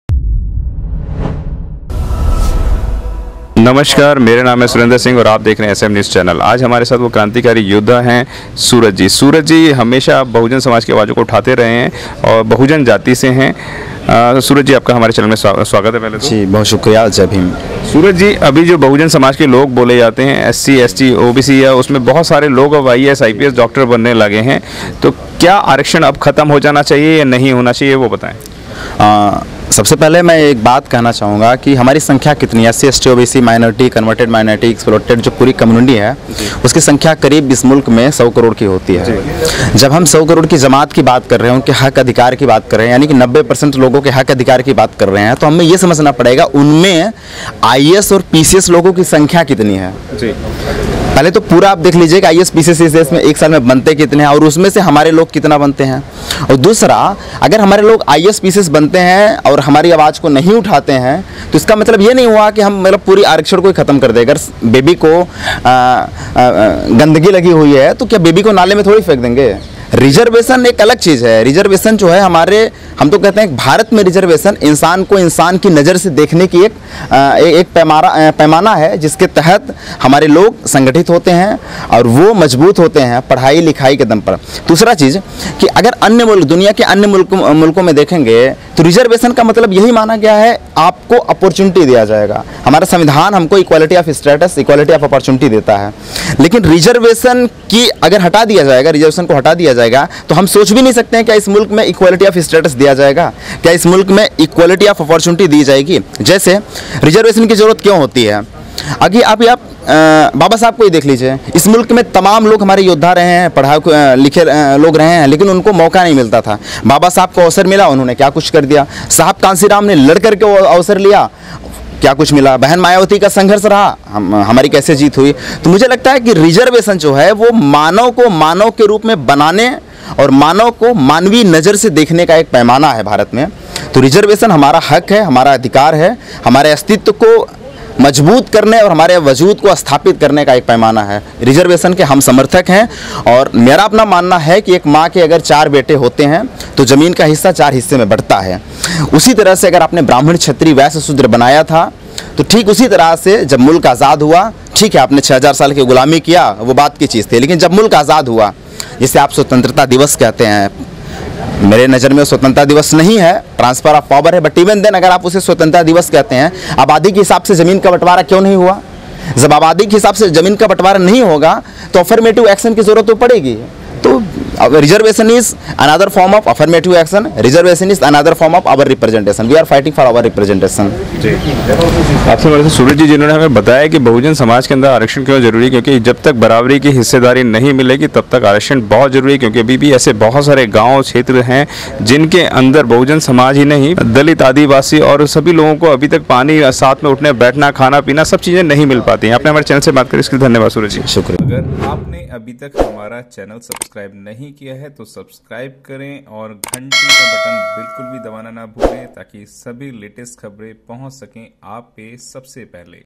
बहुजनो को आरक्षण मिलना चाहिए या नहीं , सूरज बौद्ध/ SURAJ KUMAR BAUDDH ON RESERVATION
aarakshan cast,

aarakshan movie download,

Aarakshan in hindi,

aarakshan meaning,